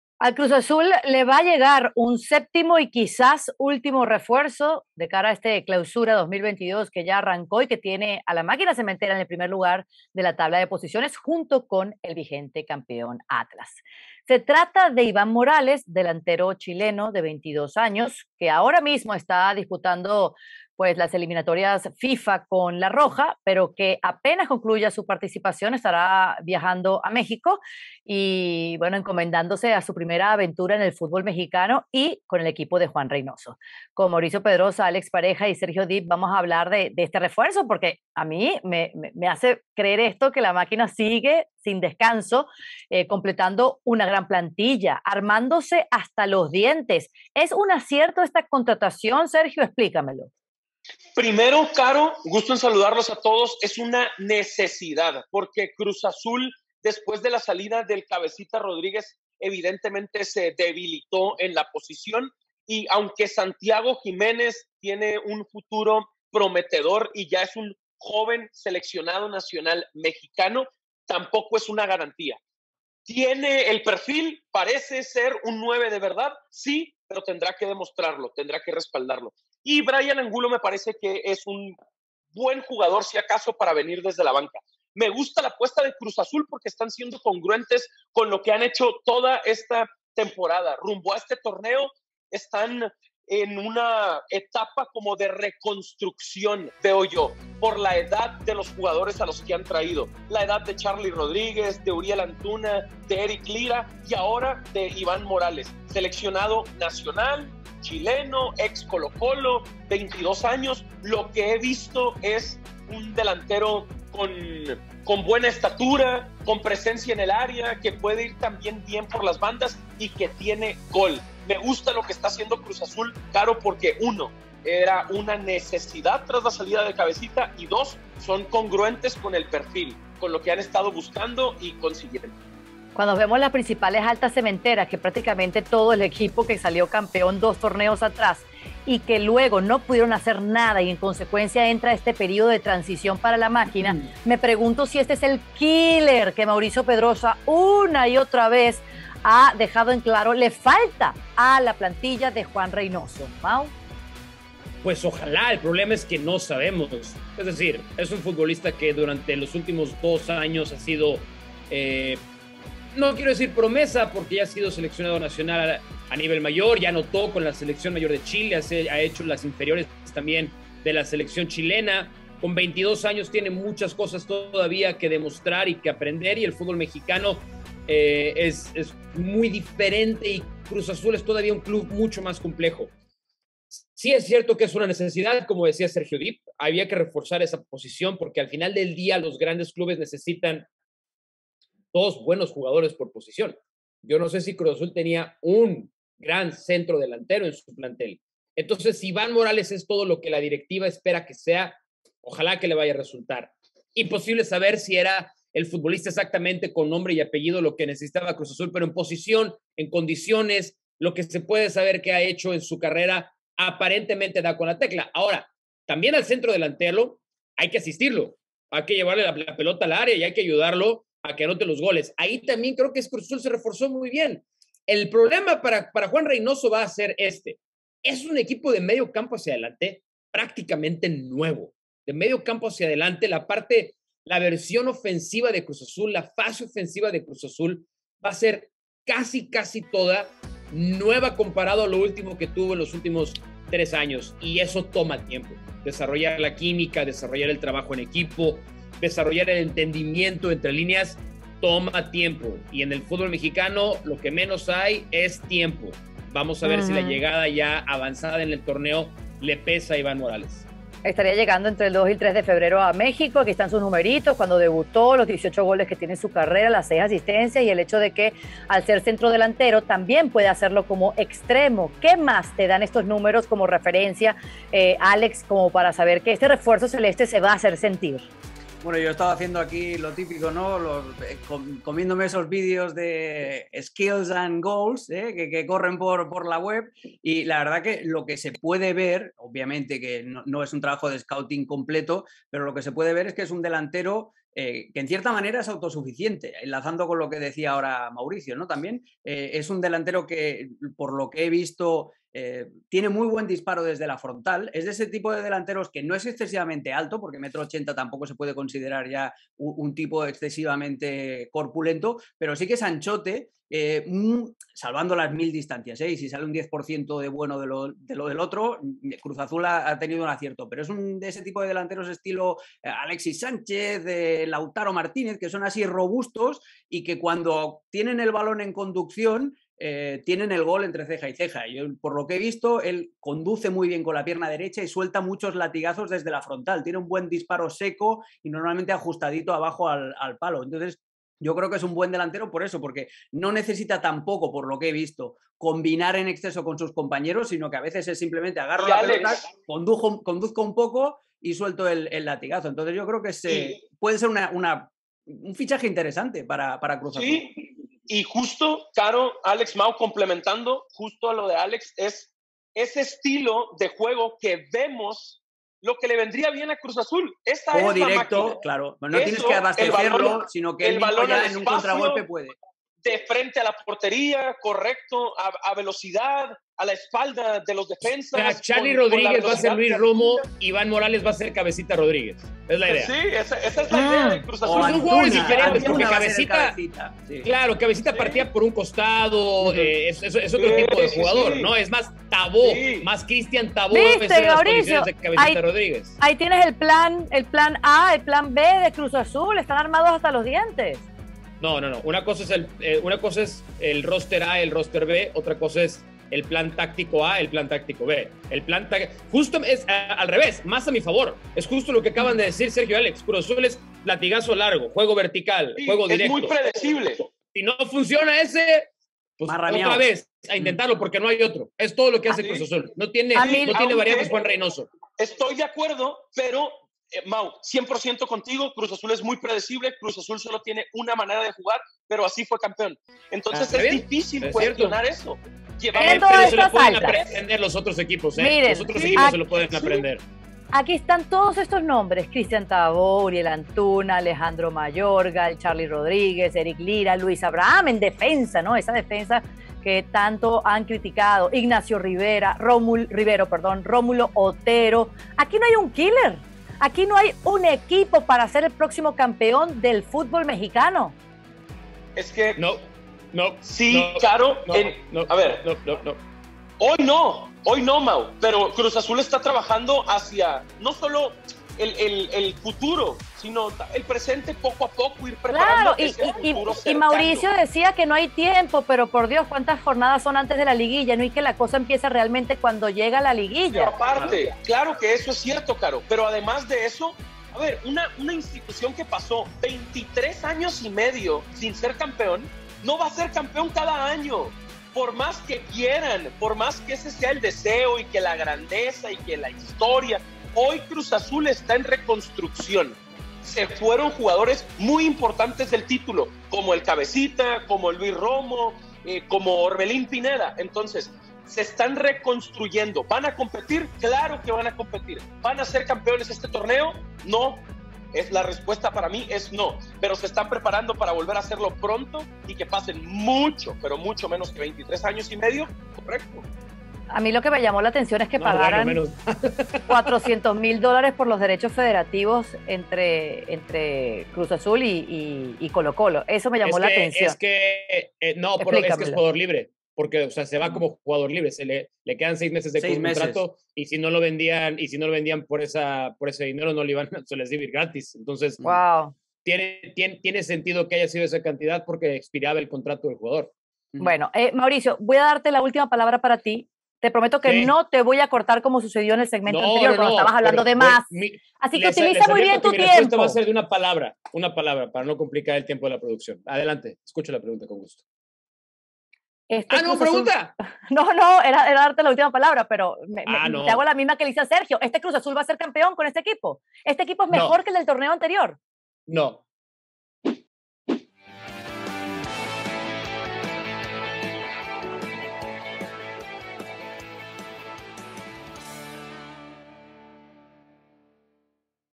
LIGA MX Nuevo REFUERZO en Cruz Azul: Delantero chileno Iván Morales LLEGA a La Máquina | Exclusivos
Nuevo refuerzo en Cruz Azul. El delantero chileno Iván Morales es la nueva contratación de La Máquina, que está completando una gran plantilla, según Carolina Guillén. Sergio Dipp calificó de necesidad la llegada de Morales al equipo. Mauricio Pedroza opinó que Morales tiene mucho que demostrar, más en la Liga MX. Alex Pareja destacó las características de juego de Morales. Exclusivos CruzAzul LigaMX | ESPN Deportes